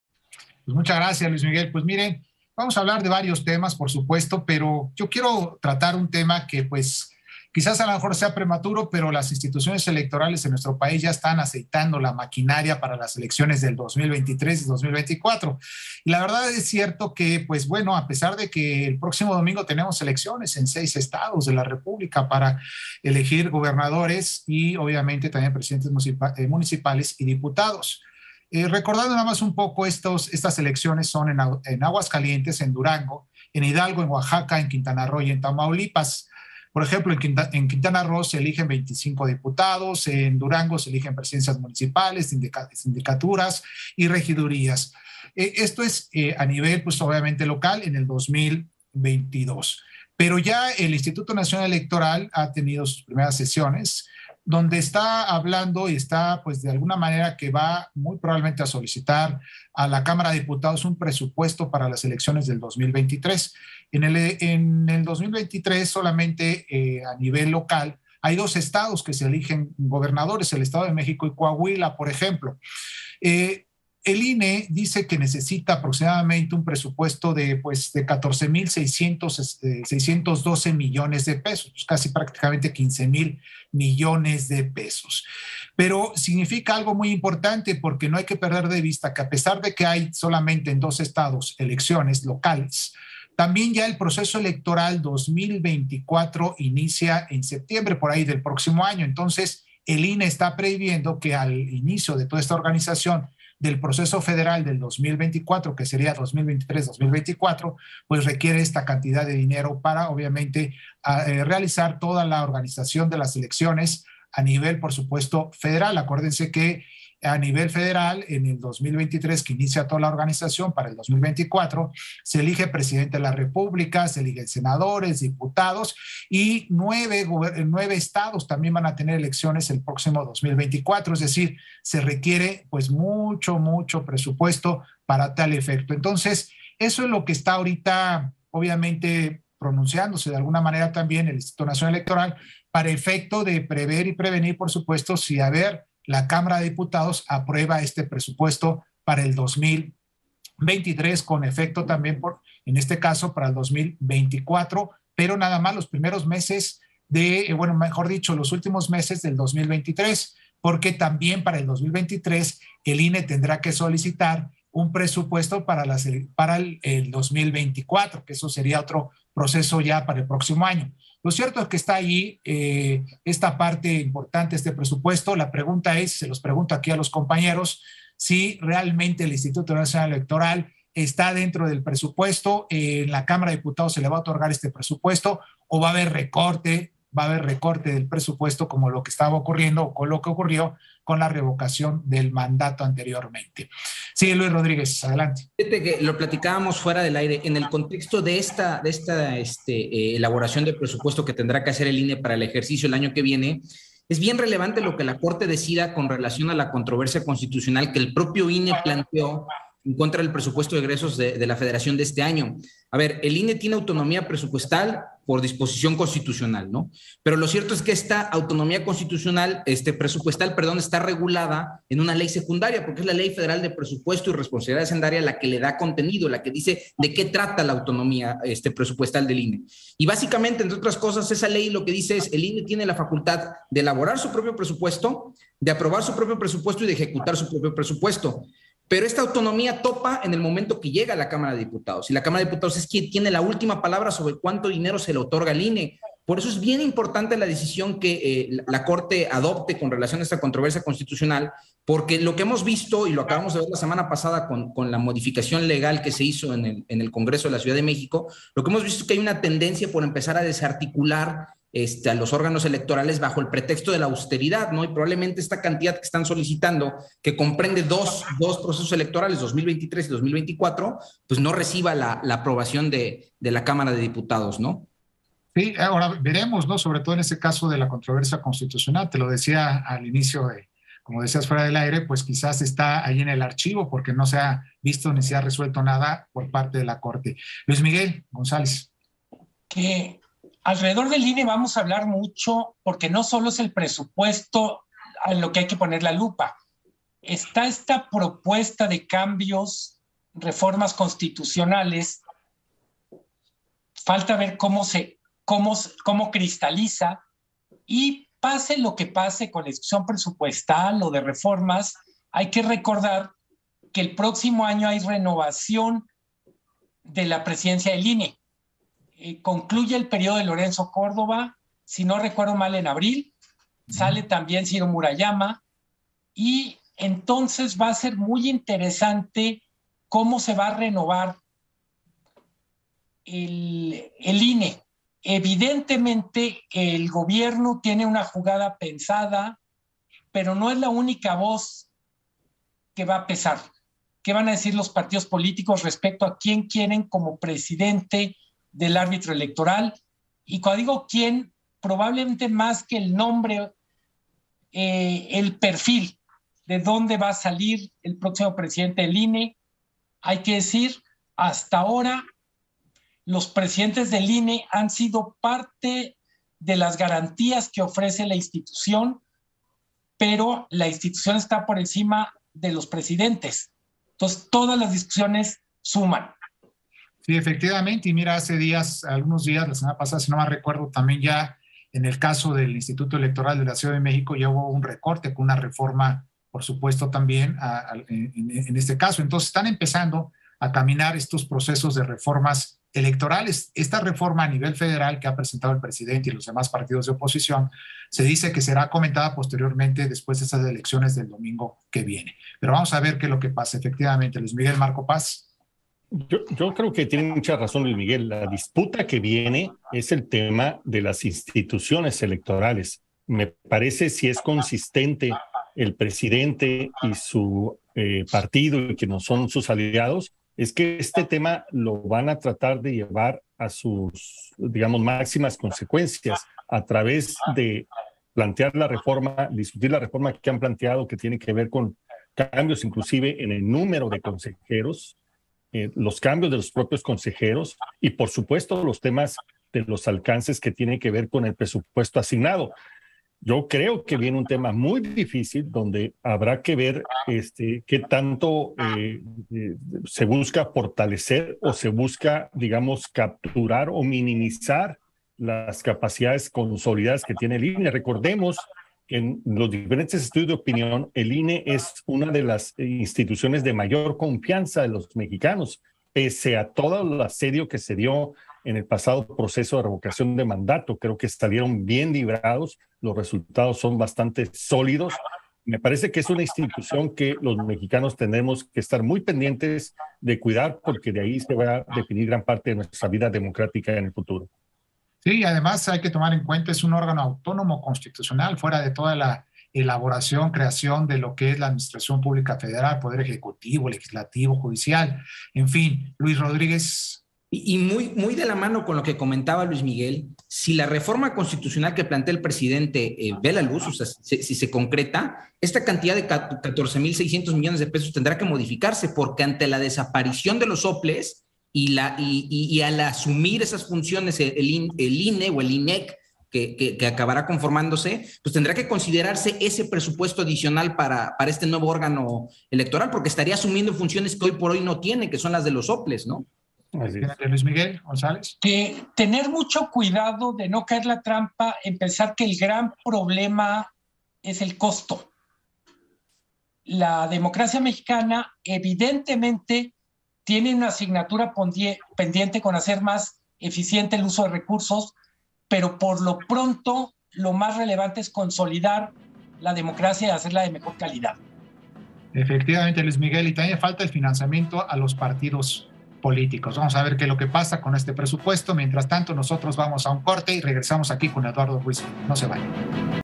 ¿El INE está dentro del presupuesto para las elecciones de 2023 y 2024 o sufrirá recorte?
Con las elecciones del próximo 5 de junio a la vuelta de la esquina, los institutos electorales del país se empiezan a preparar para las elecciones del 2023 y 2024.
La pregunta es si el INE está considerado dentro del presupuesto que asigna la Cámara de Diputados o habrá recortes como han venido haciendo desde la revocación de mandato.

Grupo Fórmula